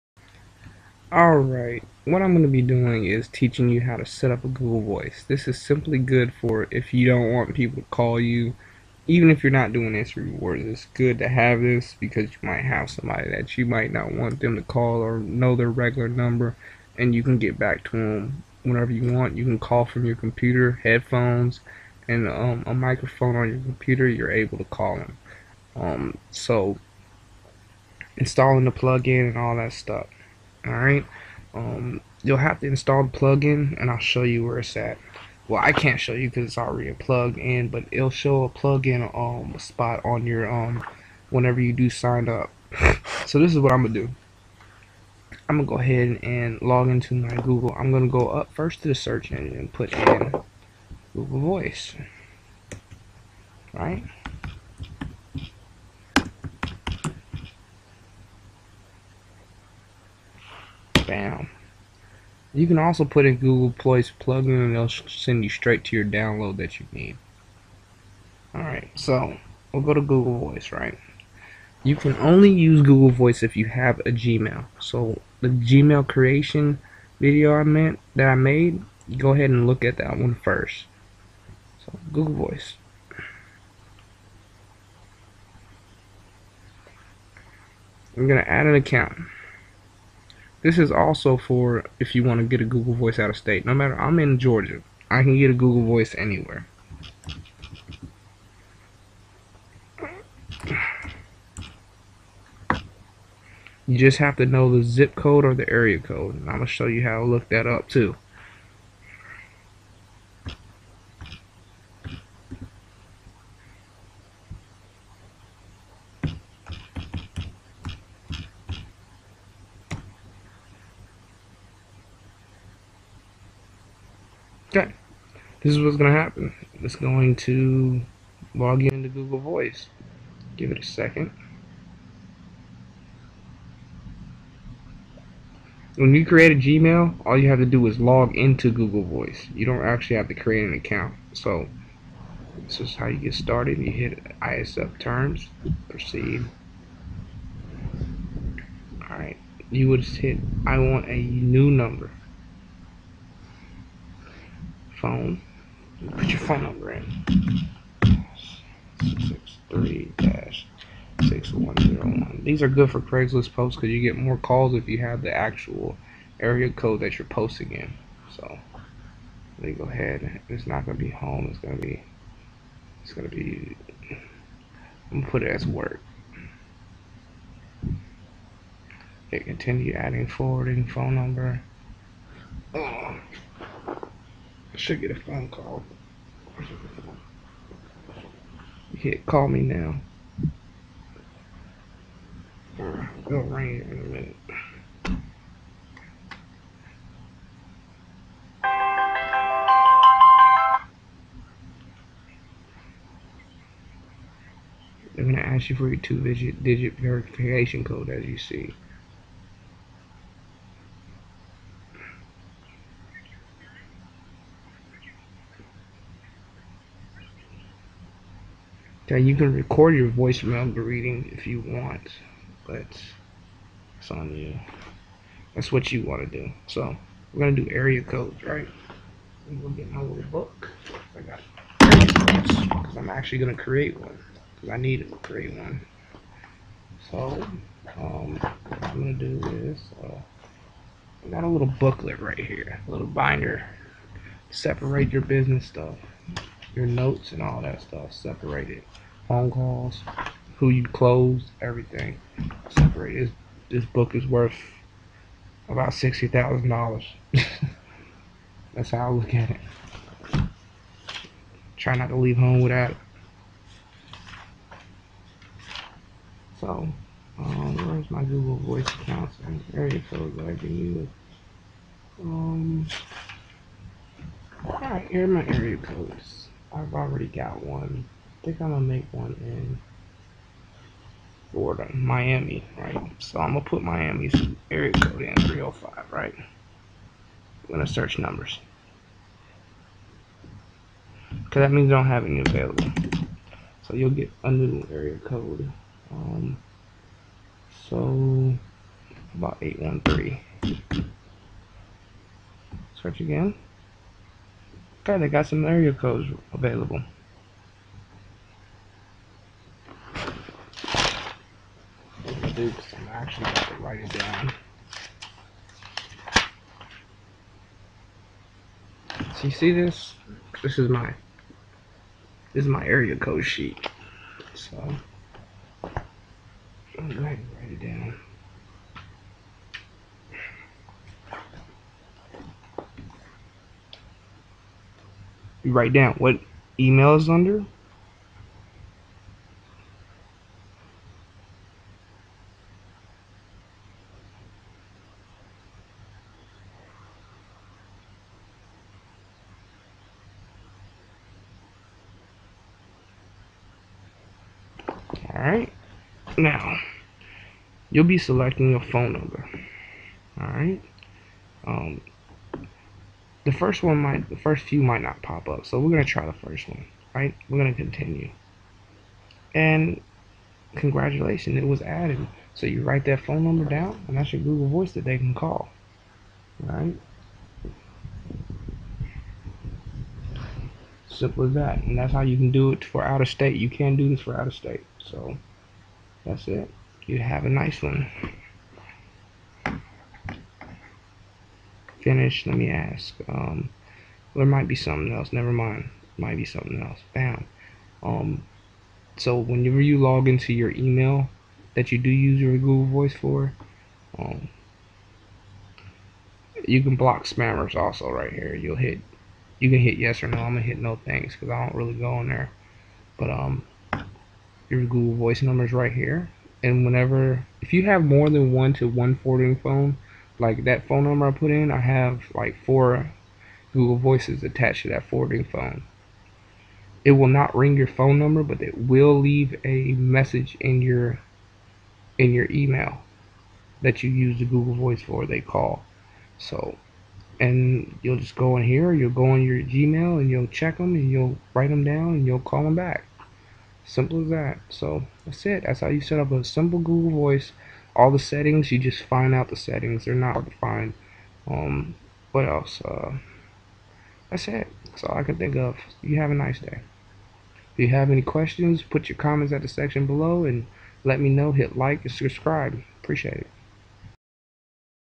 Google Voice How to setup a out of state number
This is how you setup a out of state google voice number.For more about instant Income Team.Please go